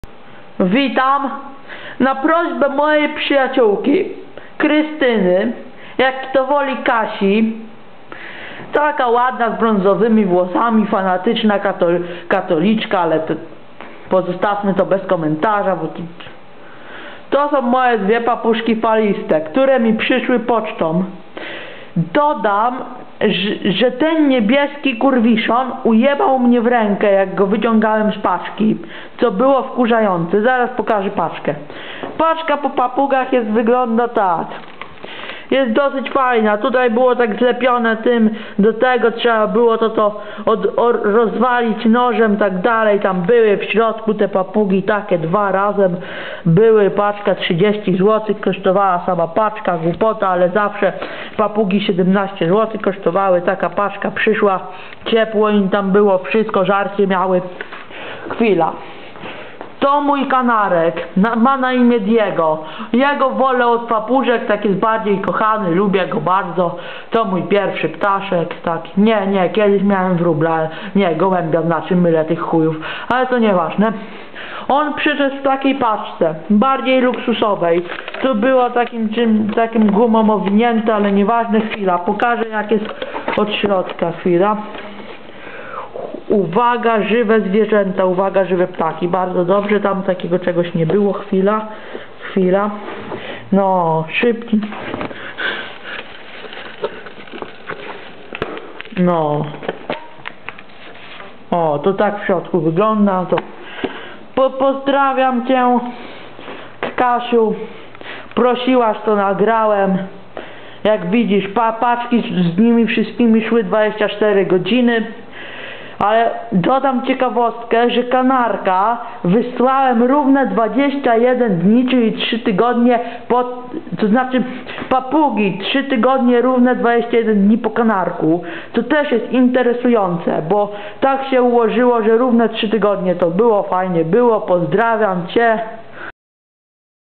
Paczki z ptakami
W takich oto paczkach przyszły moje pociechy